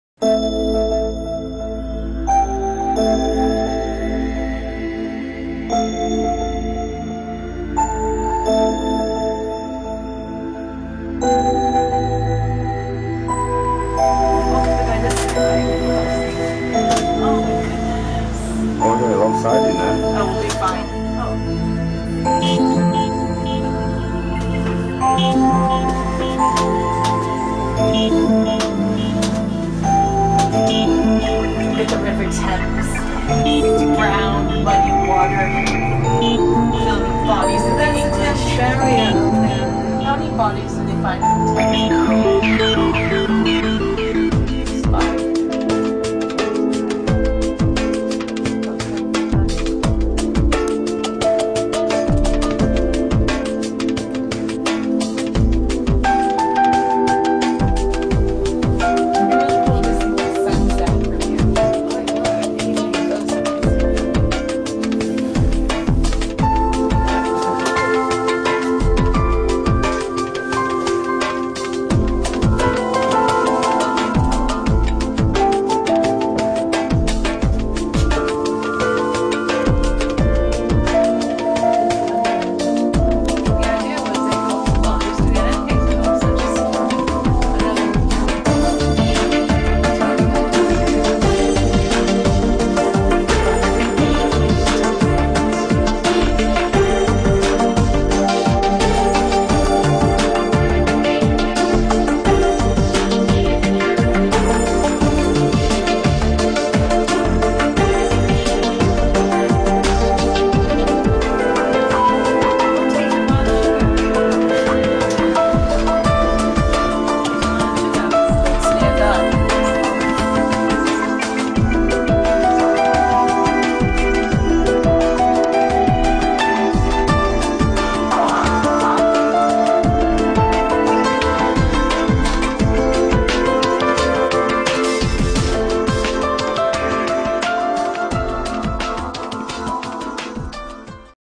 Airline Over the Thames
Emirates "Airline" over the Thames at Grenwich. Miserable December day, December 2013.